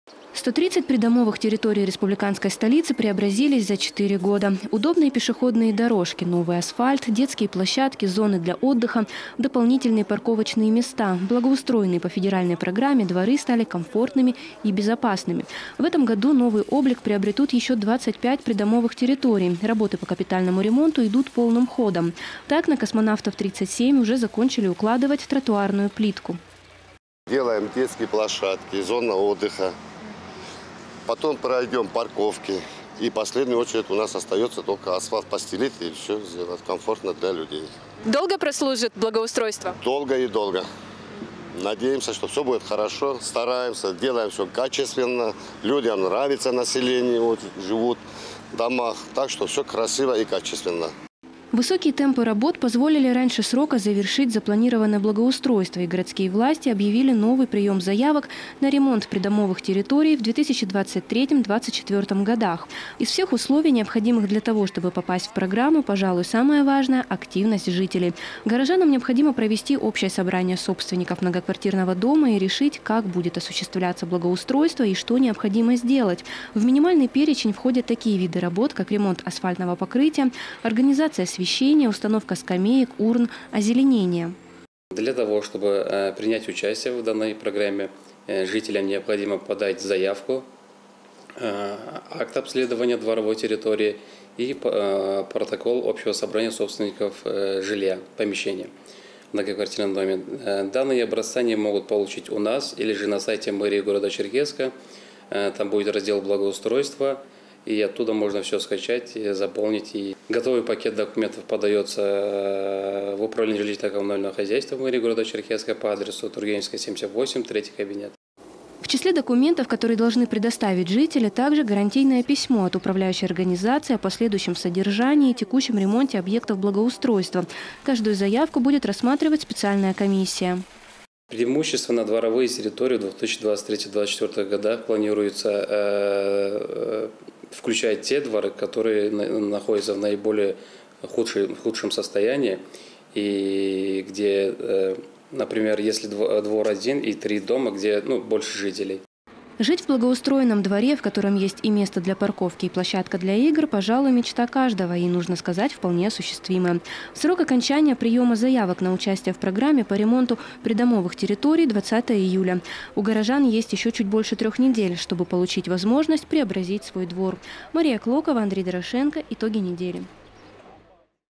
В Черкесске начался прием заявок на благоустройство придомовых территорий
В Черкесске объявлен новый прием заявок на благоустройство придомовых территорий в рамках федеральной программы «Формирование комфортной городской среды». За 4 года ее реализации в столице Карачаево-Черкесии было капитально отремонтировано 130 придомовых территорий. В этом году будет благоустроено еще 25. Срок приема новых заявок – до 20 июля.